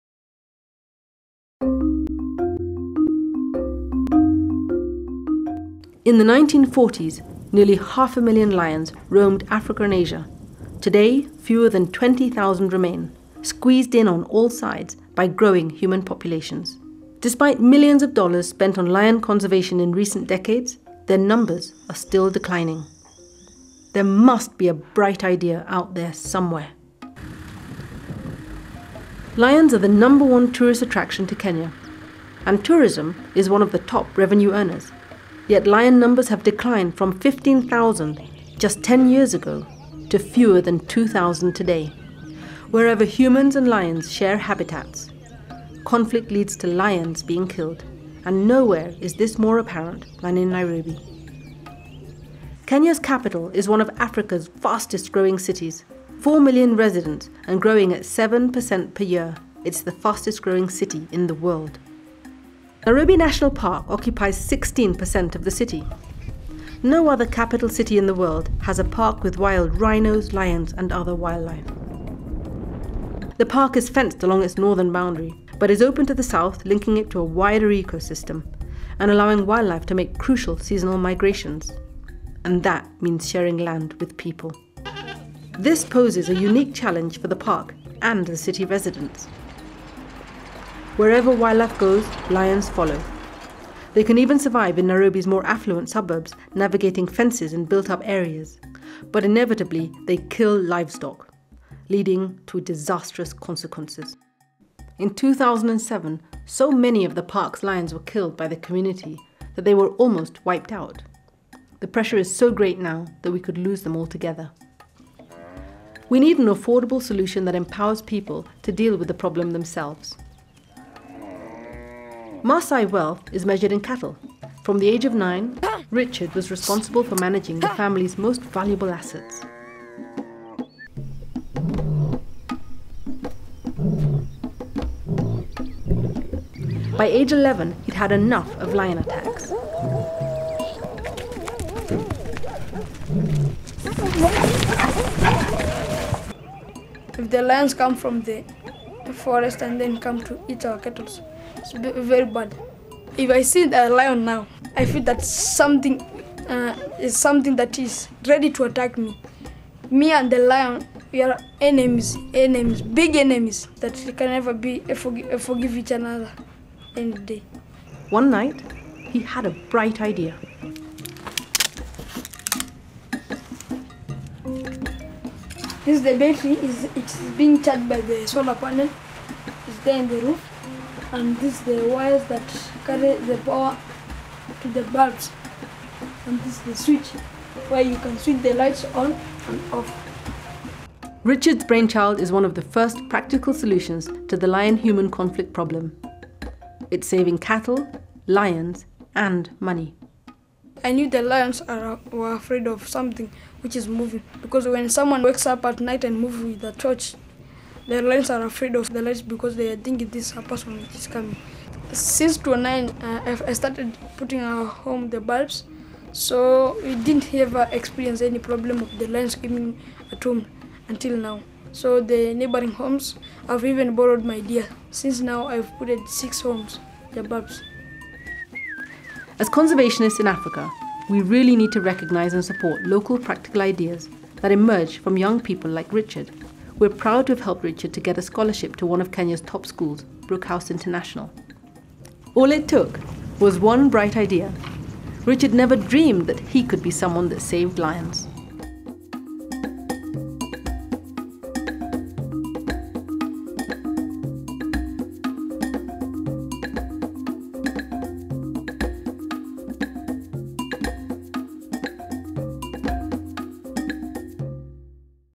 Lion Lights smoothed-H264 for PC playback.mov
13 year old Kenyan innovator Richard Turere saves endangered lions with his lion lights invention